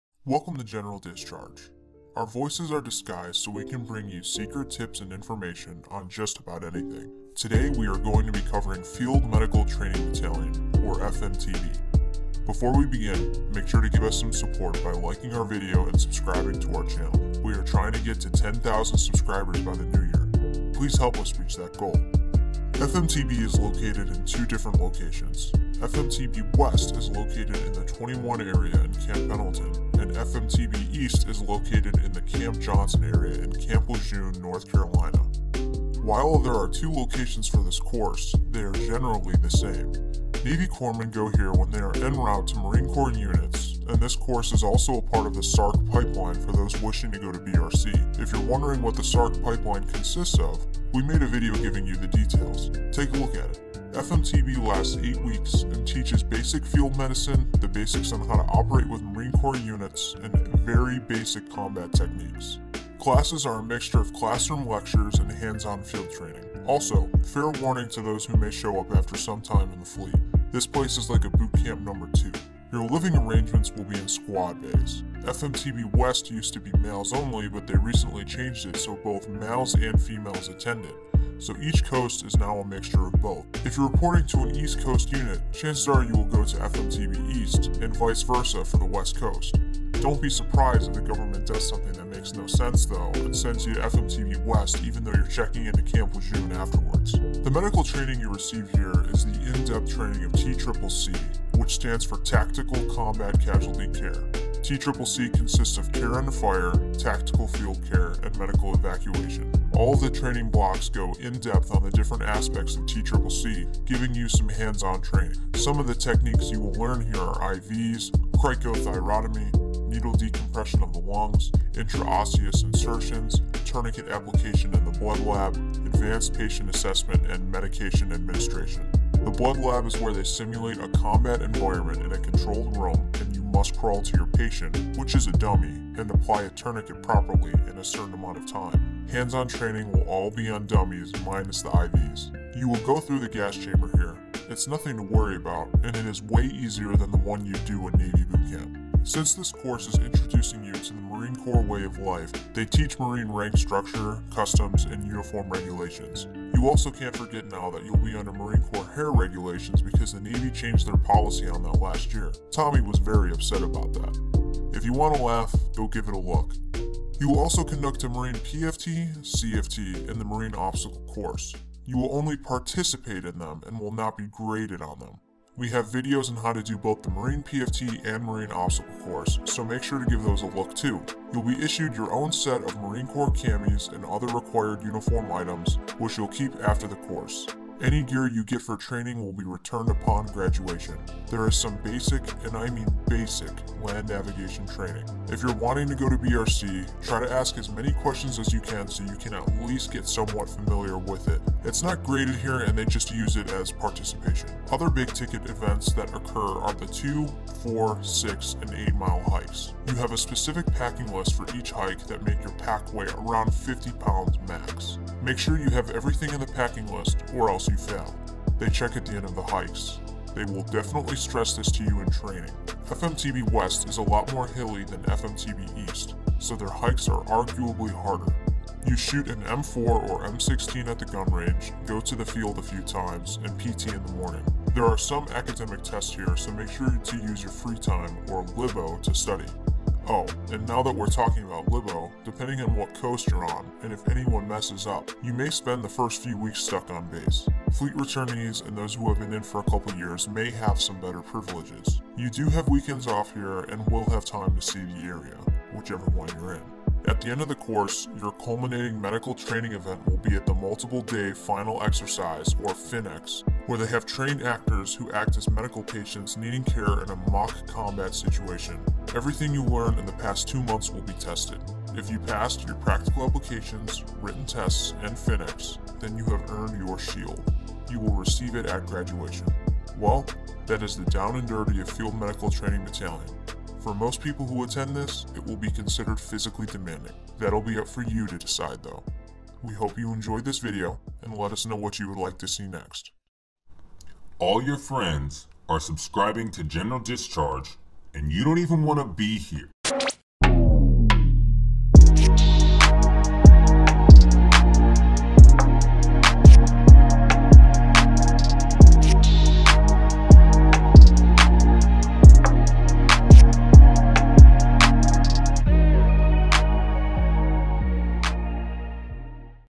FIELD MEDICAL TRAINING BATTALION (FMTB)
Welcome back to another General Discharge video! This week we are covering Field Medical Training Battalion, or FMTB. FMTB is located at Camp Pendleton, CA and Camp Lejeune, NC. All corpsman who will be assigned to Marine Corps units will go through this course, meaning that this is a part of the SARC pipeline for you Recon hopefuls out there. Enjoy!

Brands we support at General Discharge